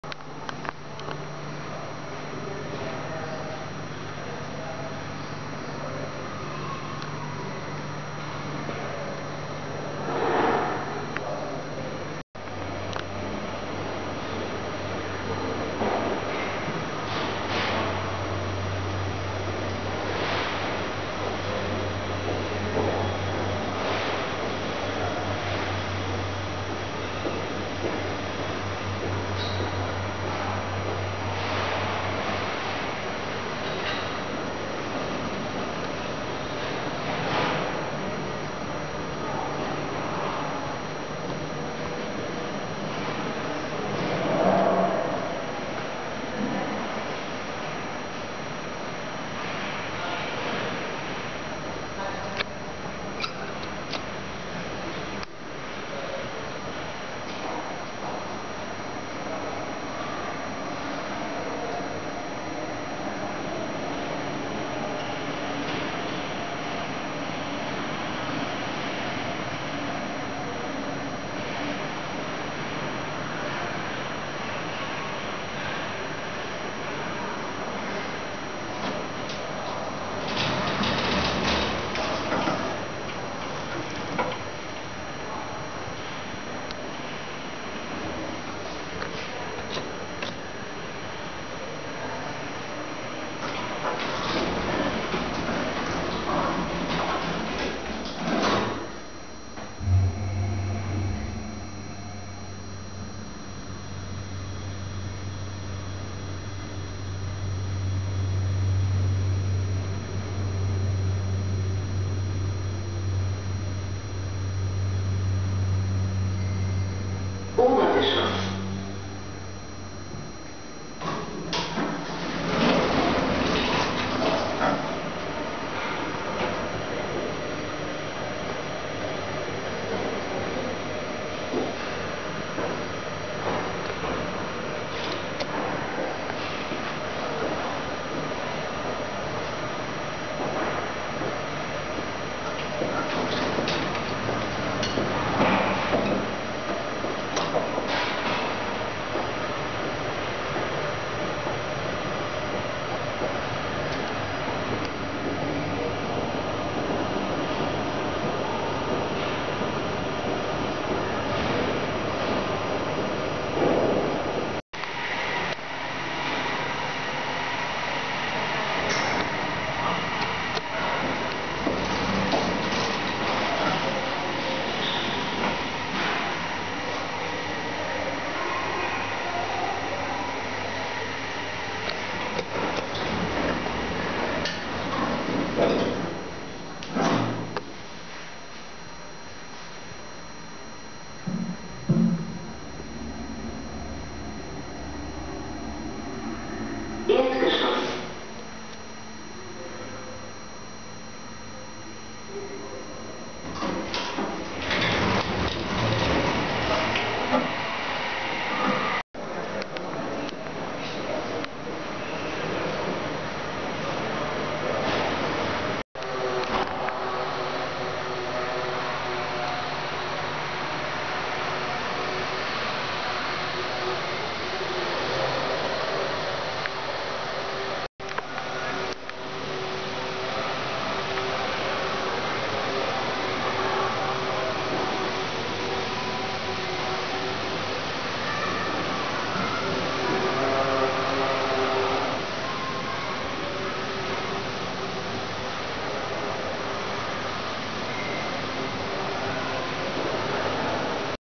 Thyssen scenic hydraulic elevator at the SI Centrum in Stuttgart
The SI Centrum in Stuttgart is an entertainment center and home to two musical theaters, as well as restaurants, hotels, and a casino. It is comprised of multiple buildings, with this one having quite an interesting architectural design. A carpeted stairway connects the ground floor to the basement and a marble staircase leads to the upper floor. Besides that, an escalator goes from the basement to the upper floor, skipping the ground floor. There are also stairs that lead down from the basement, presumably to parking levels.

Fittingly, the elevator is also quite special, having a distinct iron-cage cab design with glass panels. Also, the design above the shaft doors mimics an old-style mechanical floor indicator. It is, however, a somewhat recent installation and not a modernization of a much older elevator. Considering the size of the cab, it is not too slow, and leveling is rather quick for a hydraulic model. Although the ground floor is the middle floor, the in-cab floor indicator is labeled "0 1 2", despite a button panel in the rear of the cab being more sensibly labeled "U" for Untergeschoss (basement), "E" for Erdgeschoss (ground floor) and "O" for Obergeschoss (upper floor).

Technical specifications:
Brand: Thyssen
Capacity: 2000kg / ~4410lbs / 26 people
Type: hydraulic (inground three-stage)
Year built: 1997
Travel distance: 3 floors